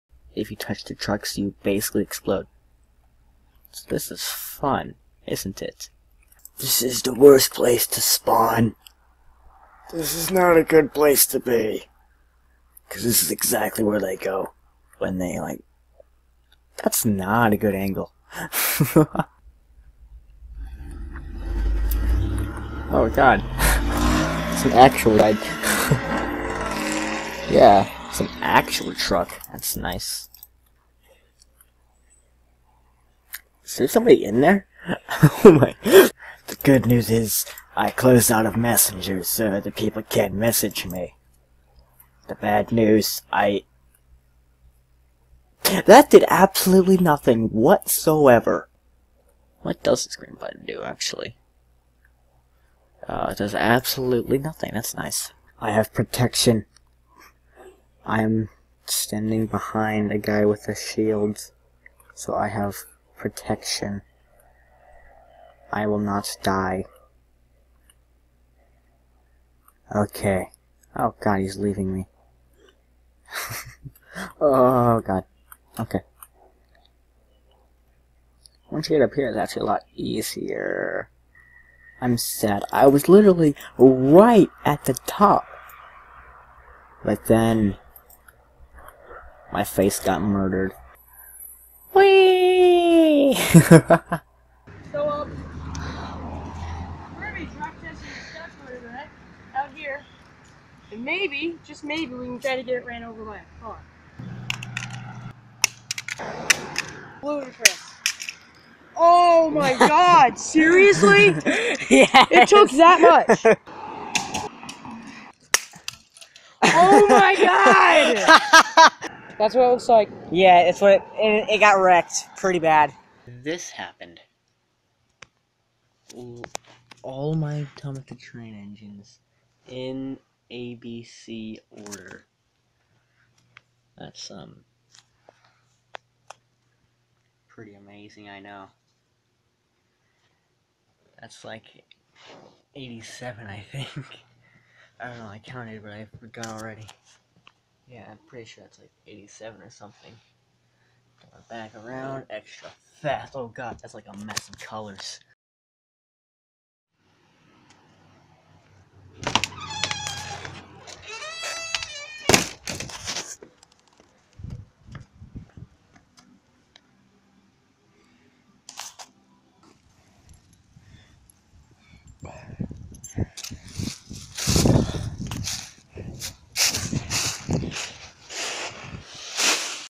Experimenting With Editing Equiptment
This is just an experimental video. I was trying to figure out how to use my editing software better. I'd say I have a decent grasp